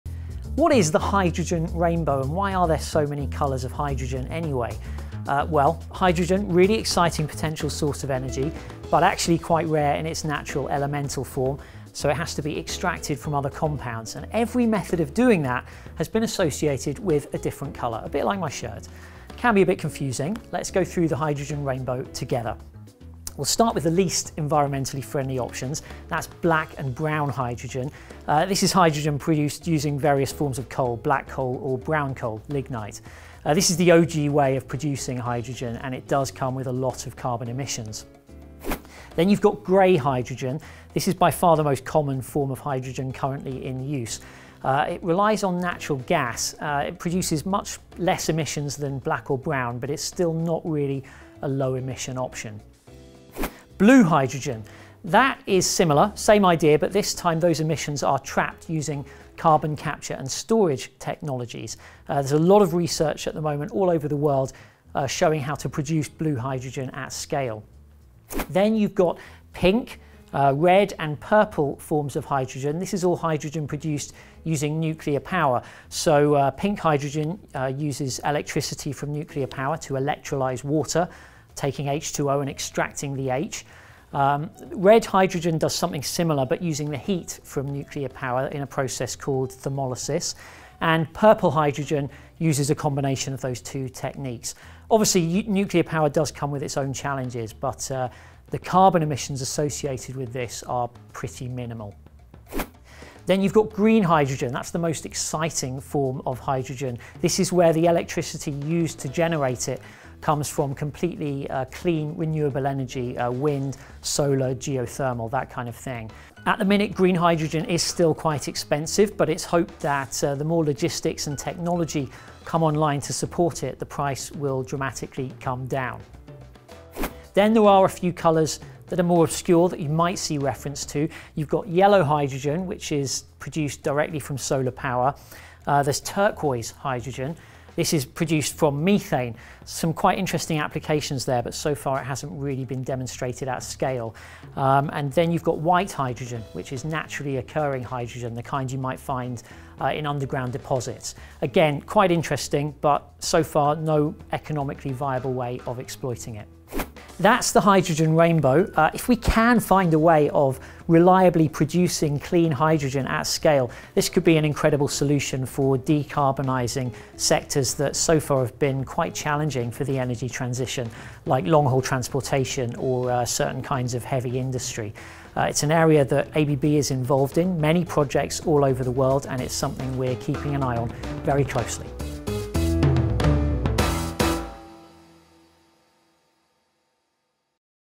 The Hydrogen Rainbow: What You Need to Know?
Did you know hydrogen comes in colors like pink, turquoise, green, and blue? Each one represents a different production method – and not all are equally sustainable.

Hydrogen is the most abundant element in the universe and a potential game-changer for a low-carbon future. But understanding the differences is key.

In our latest ABB Explainer, we break down the hydrogen rainbow and explore how each type impacts sustainability, energy efficiency, and innovation.